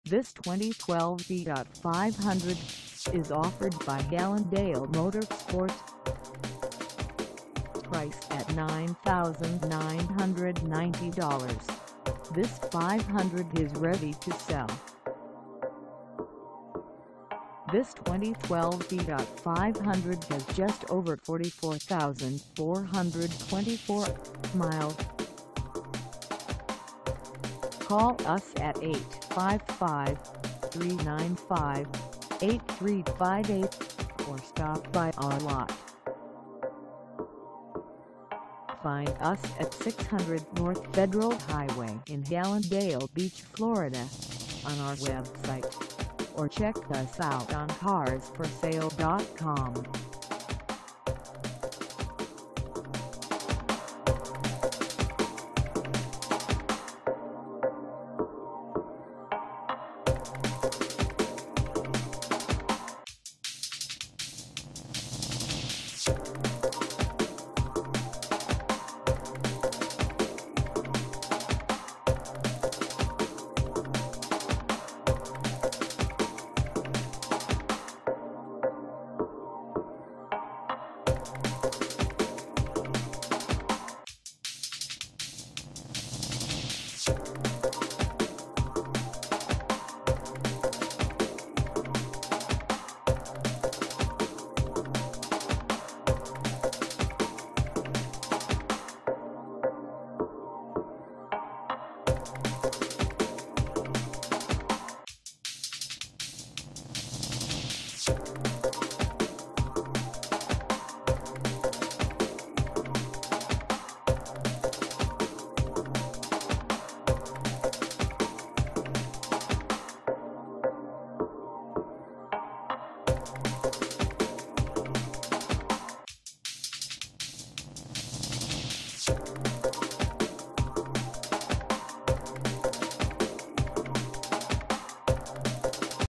2012 Fiat 500 Sport 2dr Hatchback for sale in Hallandale Bea
This 2012 Fiat 500 Sport 2dr Hatchback is for sale in Hallandale Beach, FL 33009 at Hallandale Motorsports.
Engine: 4-Cylinder 1.4L
Transmission: 5 Speed Manual
Color: Blue
Mileage: 44424
Contact Hallandale Motorsports 600 North Federal Highway Hallandale Beach, FL 33009 to test drive this 2012 Fiat 500 Sport 2dr Hatchback today.